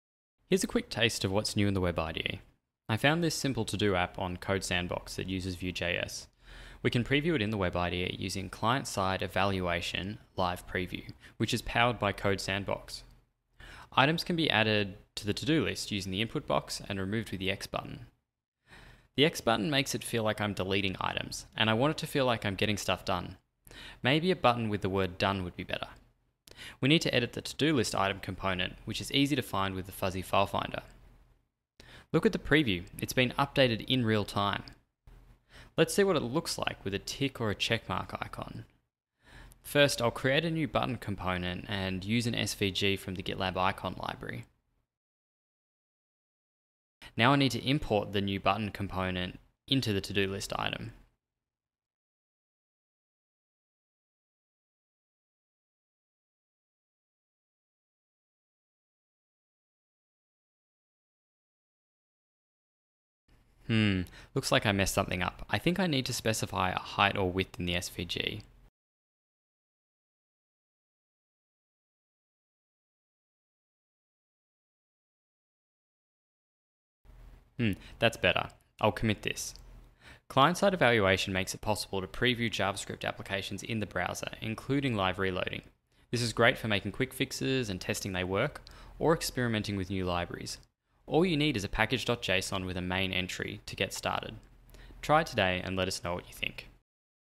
Client-side evaluation in Web IDE
In GitLab 11.2 you can preview your JavaScript web application in the Web IDE and see your changes in real time. This makes it possible to test a fix before you commit, quickly experiment with a change, or even contribute to an open source project without ever cloning it locally.